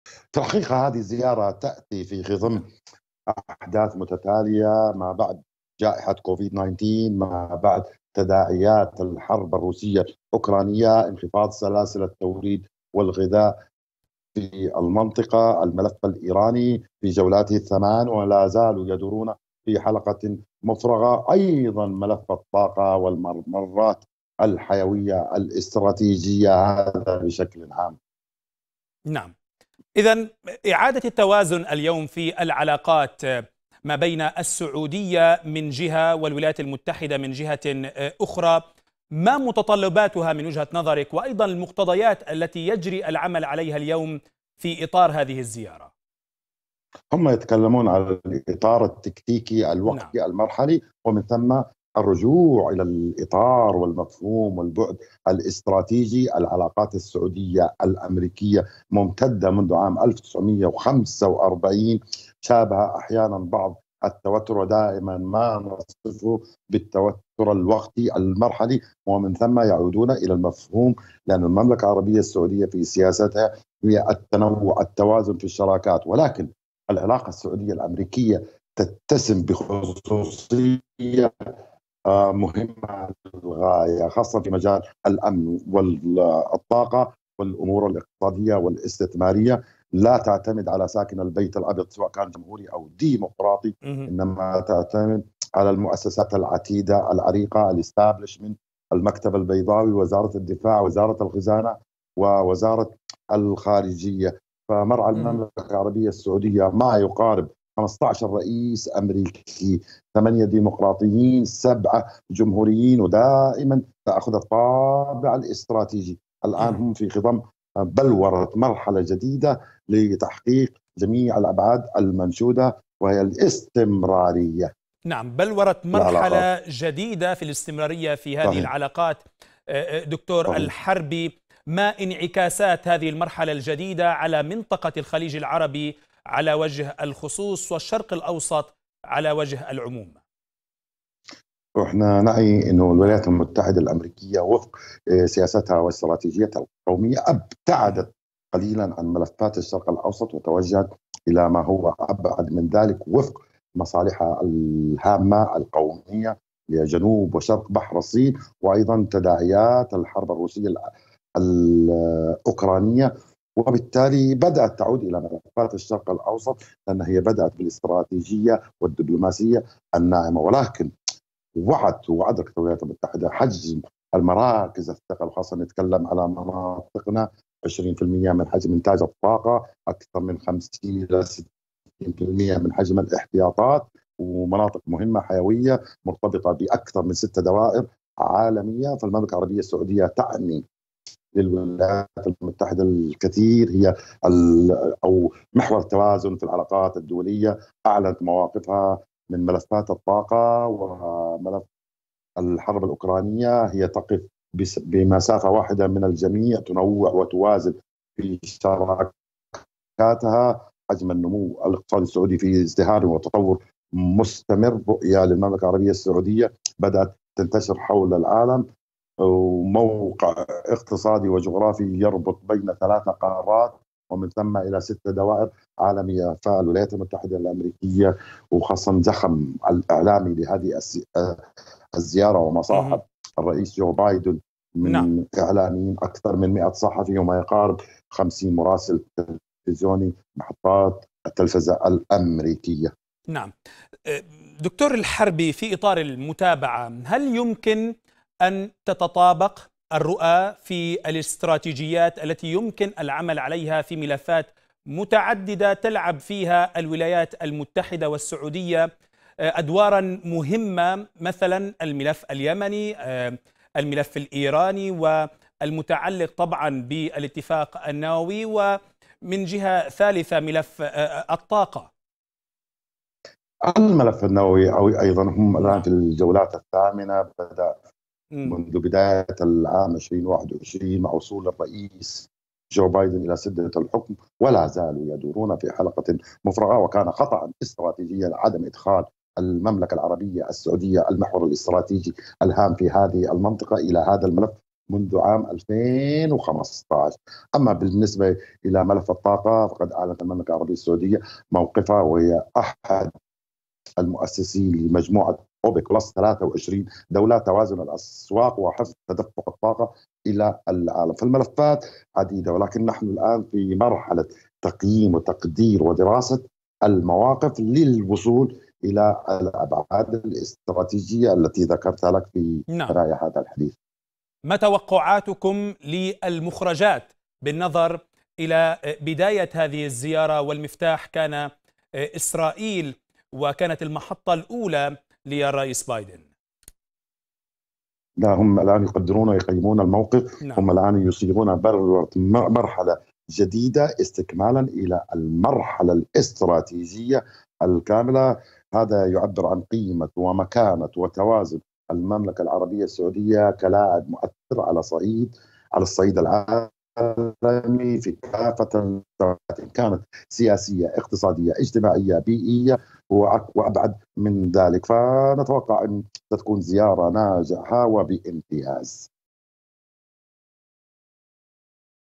باحث: زيارة بايدن إلى السعودية تستهدف بلورة مرحلة جديدة وستكون ناجحة بامتياز
وصل الرئيس الأمريكي جو بايدن إلى قصر السلام بجدة للقاء العاهل السعودي وولي العهد.
وكان في استقبال الرئيس الأمريكي ولي العهد السعودي الأمير_محمد_بن_سلمان.. ومن المقرر أن يعقد بايدن لقاء مع الملك سلمان بن عبد العزيز يعقبه لقاء مع ولي العهد..
المزيد من التفاصيل مع محمد صالح الحربي باحث في الشئون الاستراتيجية عبر النشرة الإخبارية مع الإعلامي حازم الزميلي .